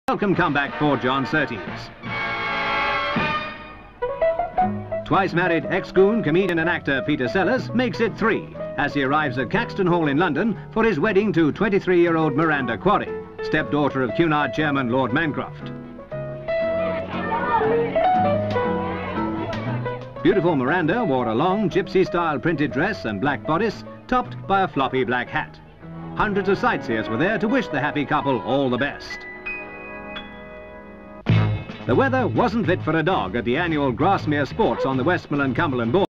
PETER SELLERS WEDDING
(27 Aug 1970) Comedian Peter Sellers takes the plunge for the third time. Crowds turn out at Caxton Hall Registry Office to see him wed beautiful Miranda Quarry, 23 year old stepdaughter of Cunard chairman Lord Mancroft.